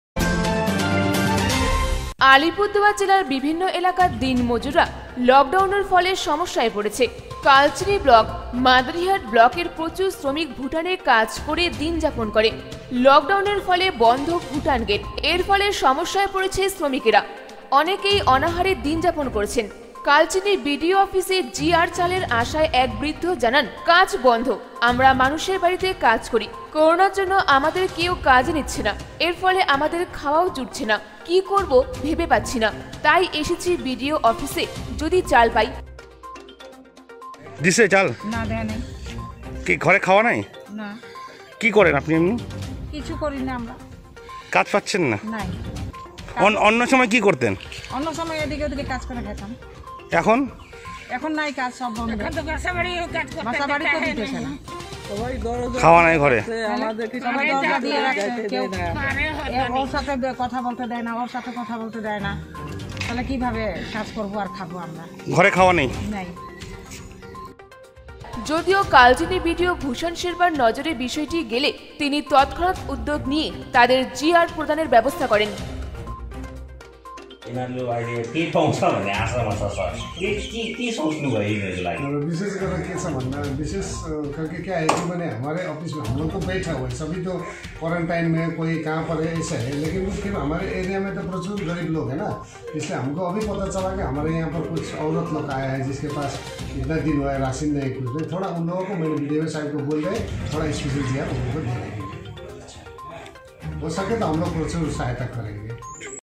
লকডাউনের জেরে সমস্যায় পড়েছে আলিপুরদুয়ার জেলার বিভিন্ন এলাকার দিনমজুররা
লকডাউনের জেরে সমস্যায় পড়েছে আলিপুরদুয়ার জেলার বিভিন্ন এলাকার দিনমজুররা,লকডাউনের জেরে ভুটান গেট বন্ধ থাকায় অনাহারে দিন যাপন করতে হচ্ছে শ্রমিকদের